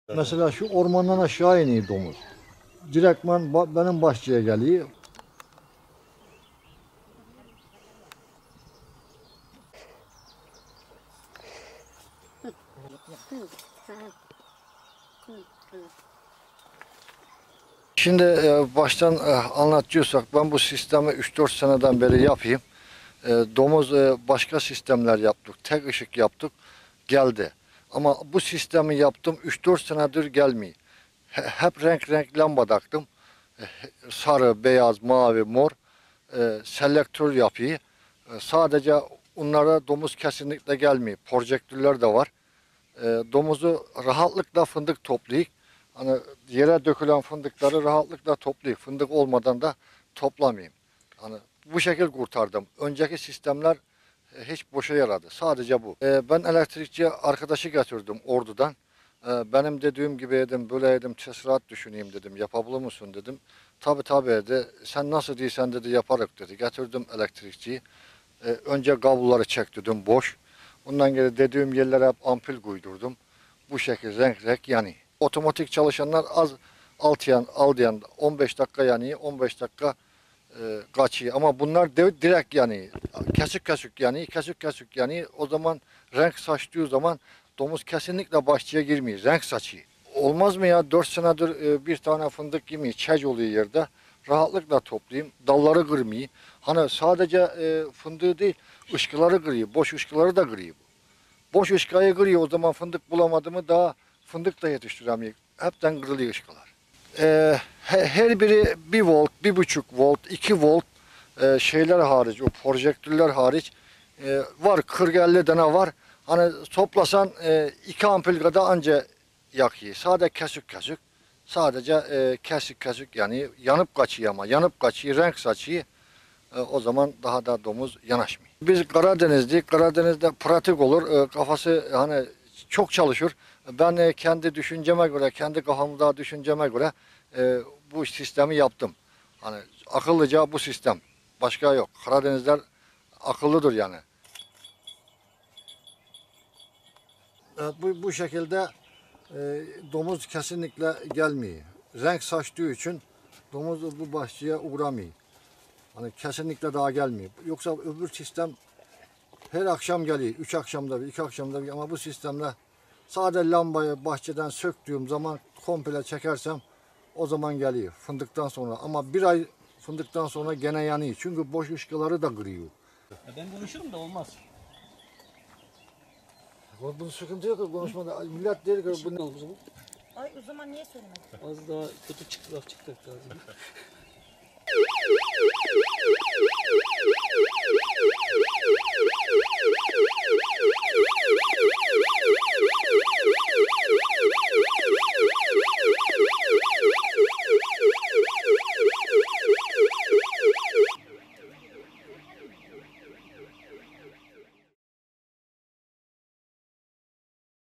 Bahçesine Dadanan Domuzlardan Bıktı... Böyle Çözüm Buldu!
Ordulu Necdet Pala, fındık bahçesine girerek zarar veren domuzlara karşı Karadeniz usulü çözüm buldu. Fındık dallarına yerleştirdiği 50 tane renkli lamba, alarm sistemi ve çakar lambalar ile 5 senedir bahçesine domuz uğramıyor.